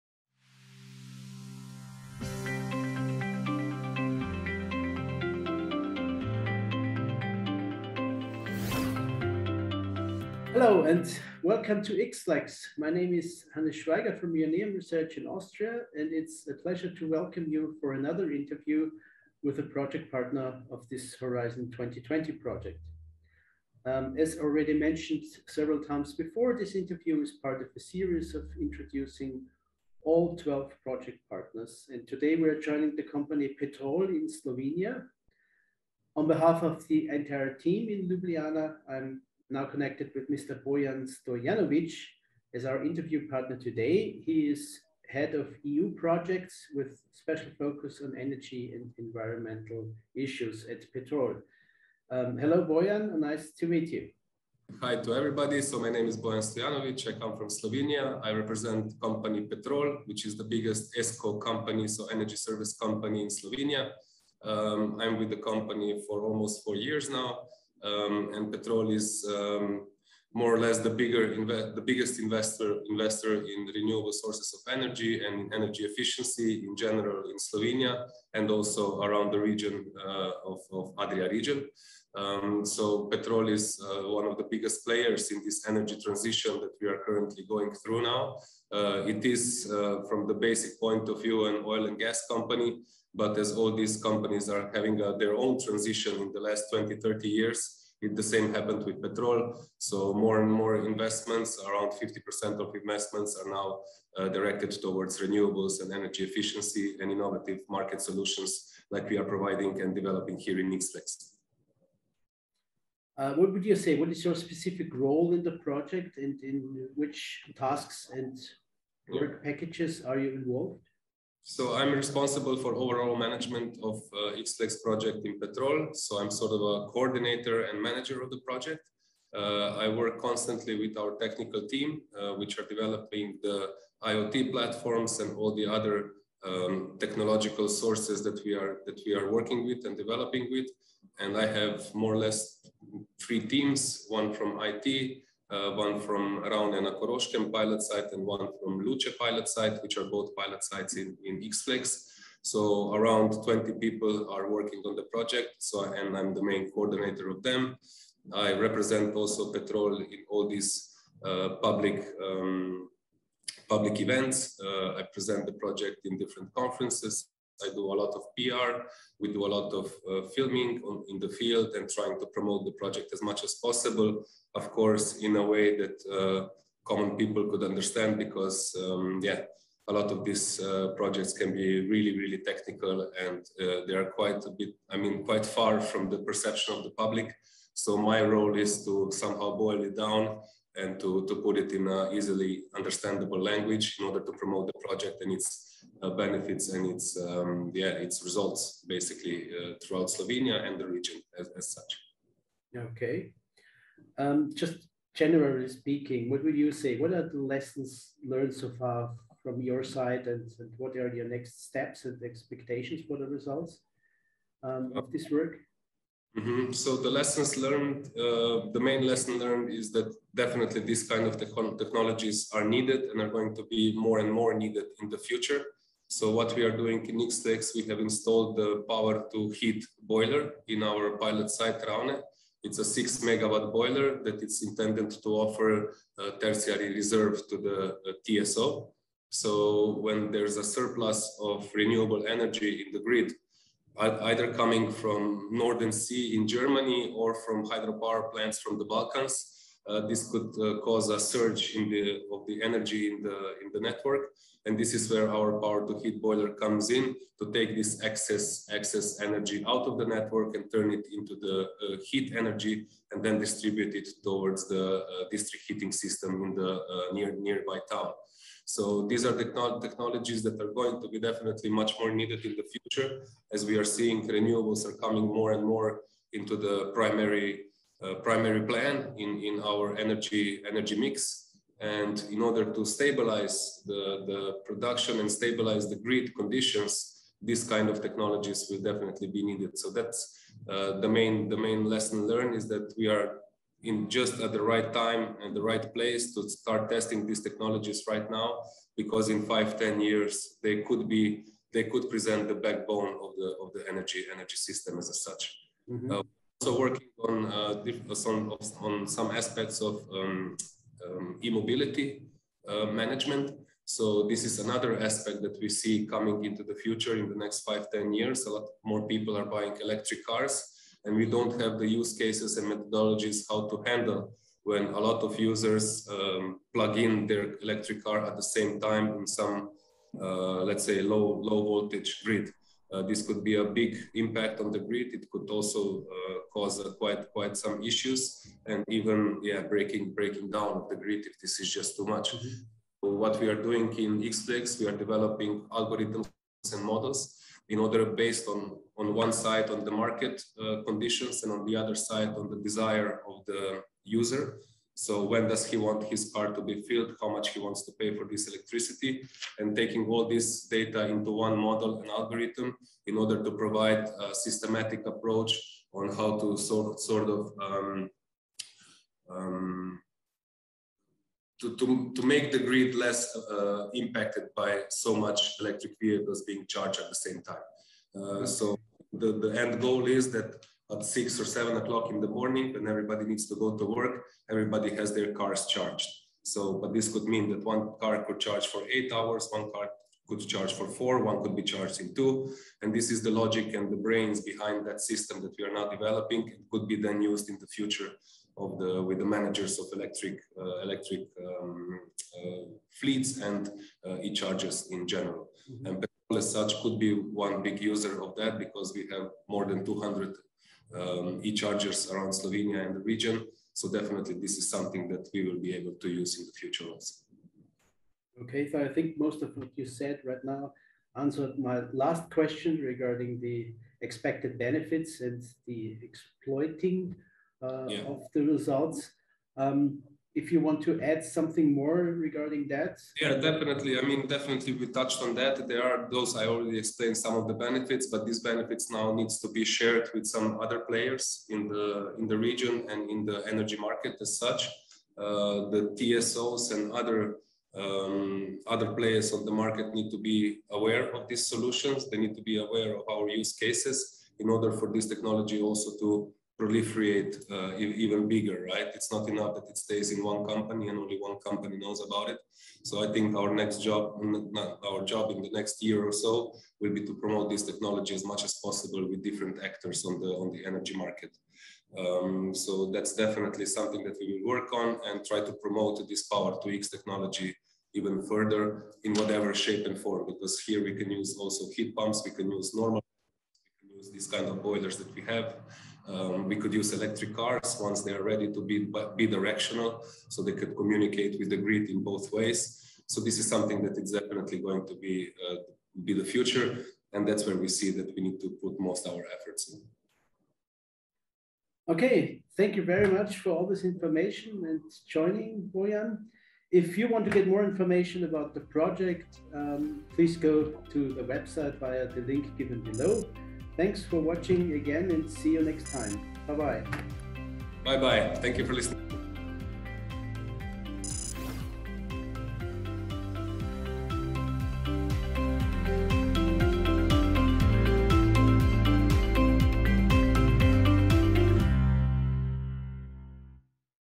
X-FLEX partner: PETROL
Meet PETROL (Slovenia) partner of the project X-FLEX in this interview with Bojan Stojanović.

PETROL, SLOVENIAN ENERGY COMPANY D.D., LJUBLJANA is responsible for the Bulgarian pilot site and actively contributes to the development of the solutions which X-FLEX is planning to address.

X-FLEX project has received funding from the European Union’s Horizon 2020 research and innovation programme under the grant agreement nº 863927.